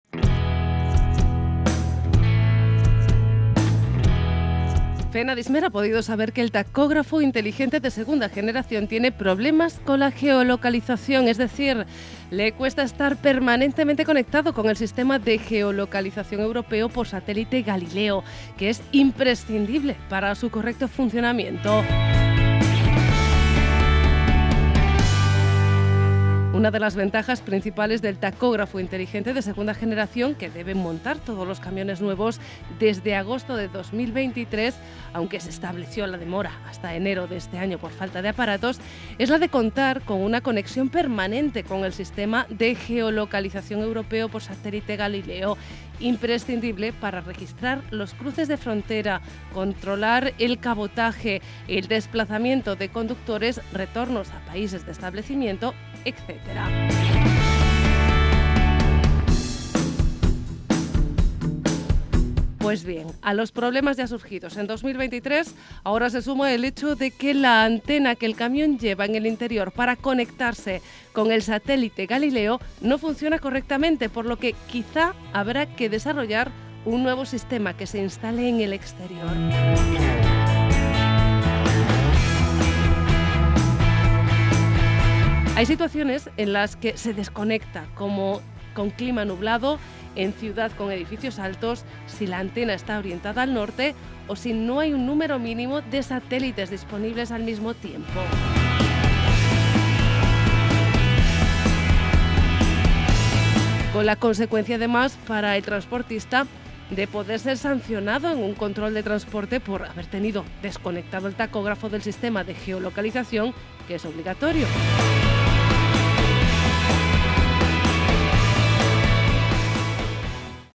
EL TACÓGRAFO DE 2 GENERACIÓN TIENE PROBLEMAS CON LA GEOLOCALIZACIÓN | By CdRas
Fenadismer ha podido saber que el tacógrafo inteligente de segunda generación tiene problemas con la geolocalización. Es decir, le cuesta estar permanentemente conectado con el sistema de geolocalización europeo por satélite Galileo, que es imprescindible para su correcto funcionamiento.
Una de las ventajas principales del tacógrafo inteligente de segunda generación, que deben montar todos los camiones nuevos desde agosto de 2023, aunque se estableció la demora hasta enero de este año por falta de aparatos, es la de contar con una conexión permanente con el sistema de geolocalización europeo por satélite Galileo. Imprescindible para registrar los cruces de frontera, controlar el cabotaje, el desplazamiento de conductores, retornos a países de establecimiento, et
Pues bien, a los problemas ya surgidos en 2023, ahora se suma el hecho de que la antena que el camión lleva en el interior para conectarse con el satélite Galileo no funciona correctamente, por lo que quizá habrá que desarrollar un nuevo sistema que se instale en el exterior. Hay situaciones en las que se desconecta, como clima nublado, en ciudad con edificios altos, si la antena está orientada al norte o si no hay un número mínimo de satélites disponibles al mismo tiempo.
Con la consecuencia, además para el transportista de poder ser sancionado en un control de transporte por haber tenido «desconectado» el tacógrafo del sistema de geolocalización, que es obligatorio.